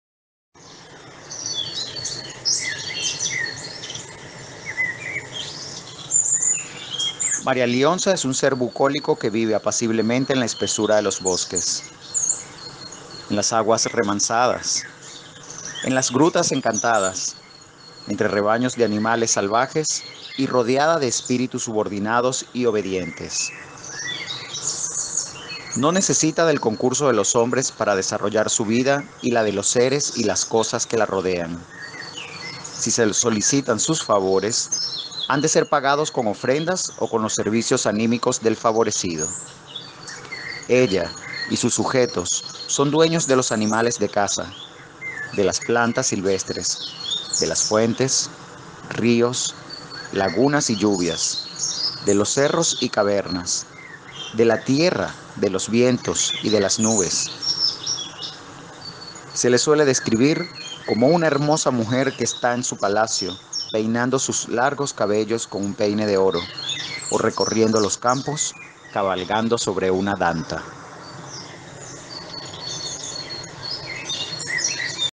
El mito de María Lionza ...por Francisco Tamayo
Tamayo, F.  (1943) Boletín del Centro Histórico Larense año II N. V enero-marzo.

MATERIAL PUBLICADO EN NUESTRA CUENTA IG @marialionzalamadre